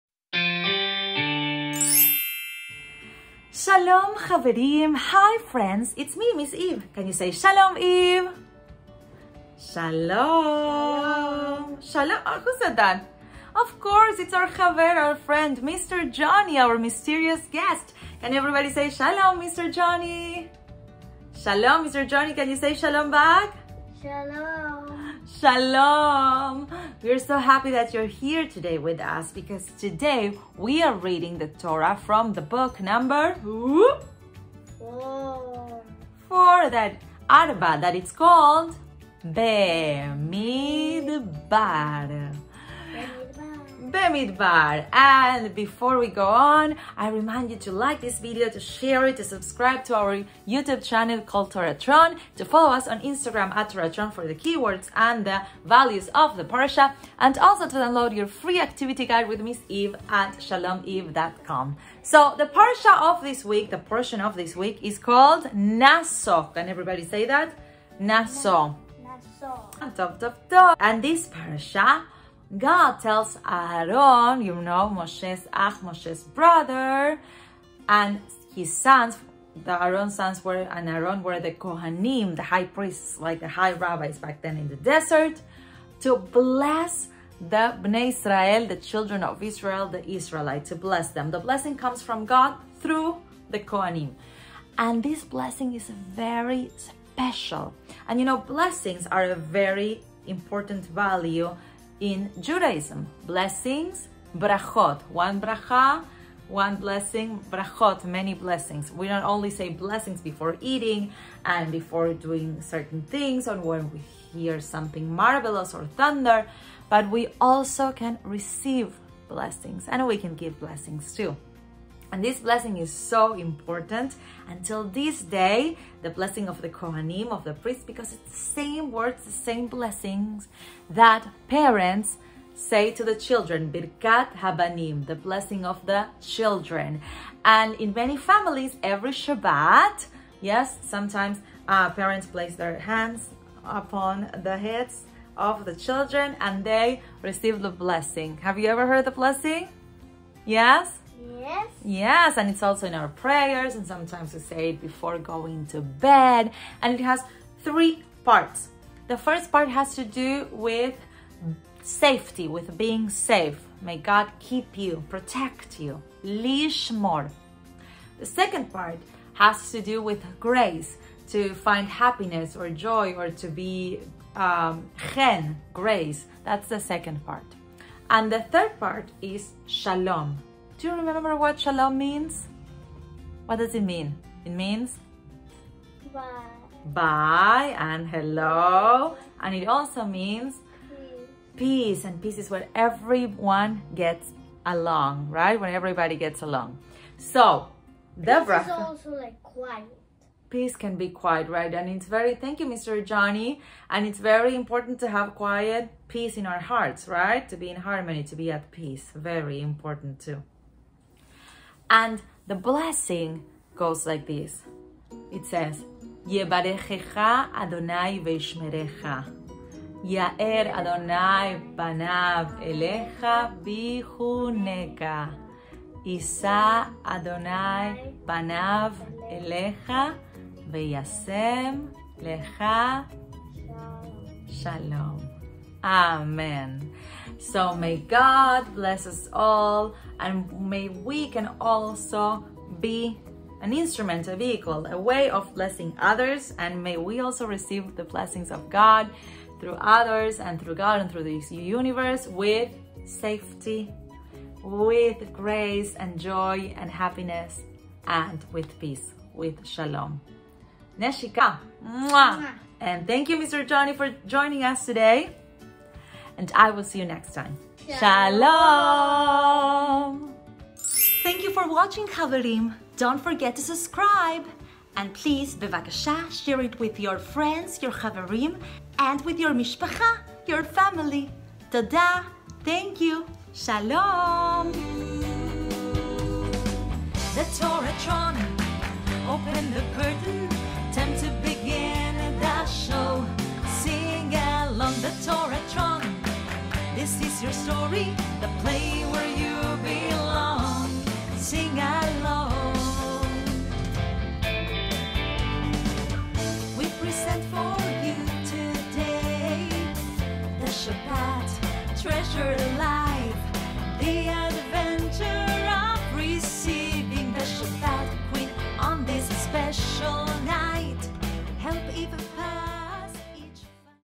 Parashah Naso with Ms. Eve
Join Ms. Eve🌟 and discover special Arachim (values) and Mitzvot in this weeks’ Parasha (Torah portion.)  Parashah NASO .   BIRKAT HABANIM (Blessing of the children). Download the FREE Parashah with Ms. Eve activity companion

Link a este video en español: